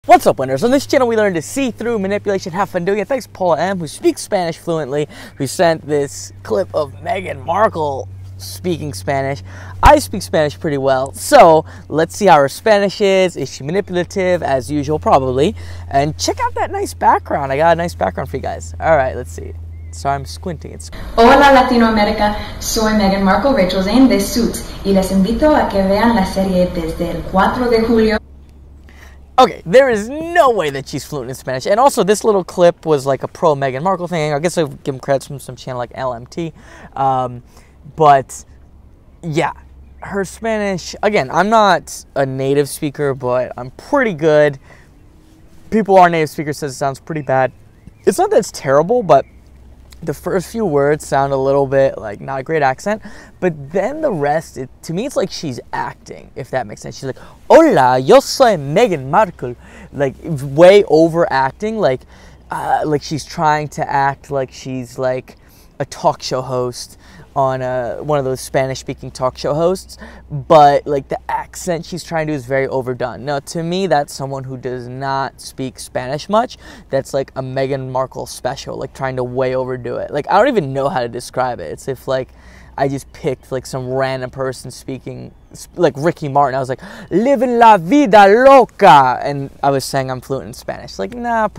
Reacting Meghan Pretends To Speak Spanish#meghanmarkle
Meghan Markle is quite the goof. According to Spanish speakers this does NOT sound like a fluent Spanish speaker. I speak a bit of spanish and Meghan Markle sounds pretty odd to me as well.